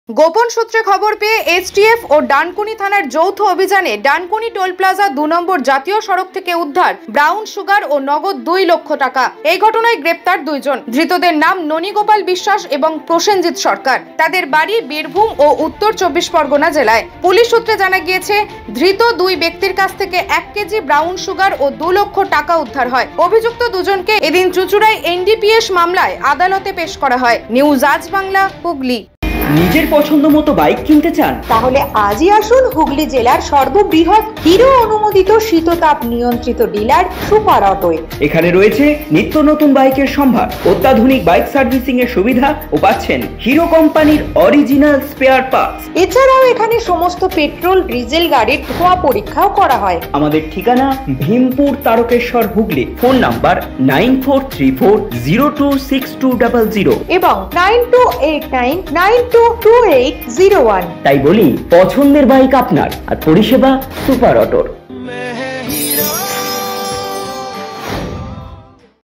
ব্রাউন সুগার ও টাকা সহ গ্রেফতার ২
এসটিএফ ও ডানকুনি থানার যৌথ অভিযানে ডানকুনি টোল প্লাজা ২ নম্বর জাতীয় সড়ক থেকে, উদ্ধার ব্রাউন সুগার ও নগদ দুই লক্ষ টাকা।